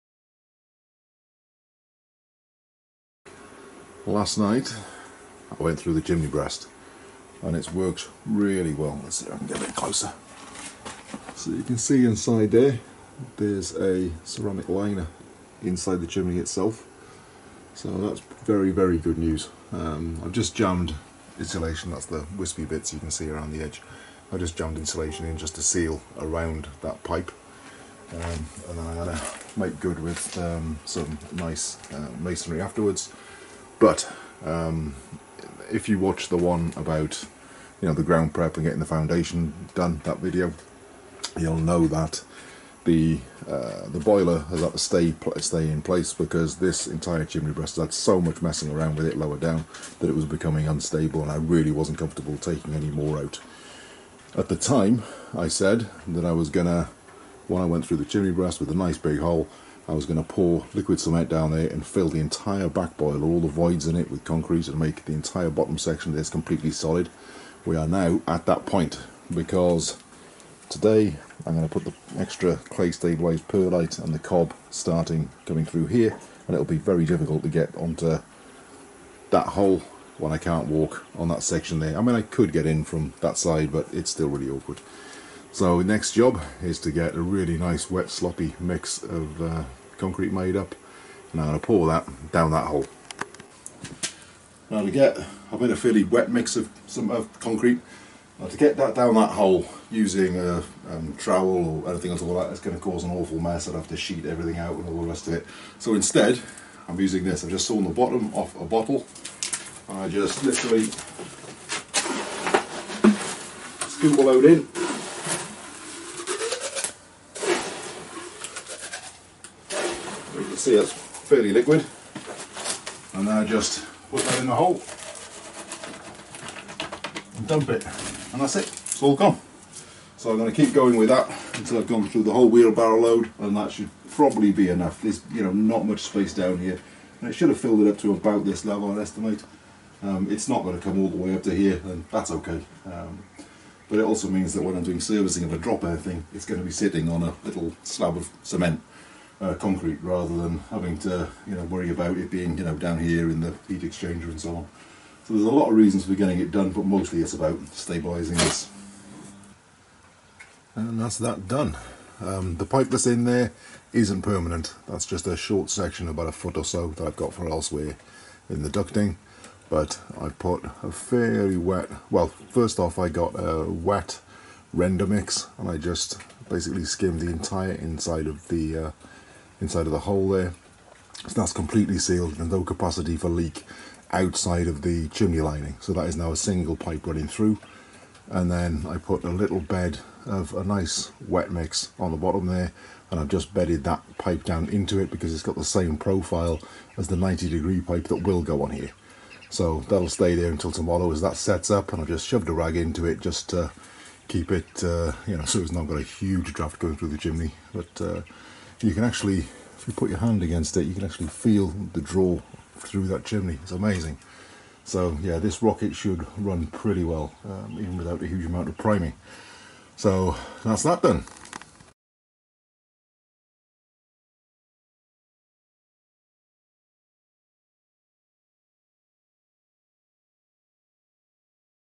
Rocket mass heater build 7. Through the chimney. The most efficient heating in the world.
This video series details our construction of the most efficient space heater in the world, the rocket mass heater.
Rocket mass heaters are incredibly efficient as space heaters.
Typically using just 10% of the fuel of conventional wood heat, zero fossil fuel inputs, no electricity needed, and no moving parts. Even using new components, a mass heater typically costs less than £1000 to build, extremely cheap when compared to well over £5000 for a conventional heating system.
Using easily sourced stick fuels, these systems can be carbon neutral, or even carbon negative. What other heating system can claim to heal ecosystems while sequestering carbon? Rocket mass heaters have the potential to revolutionise reversing climate change while giving their owners fuel independence.
In this video I splice the steel ducting into the existing chimney breast.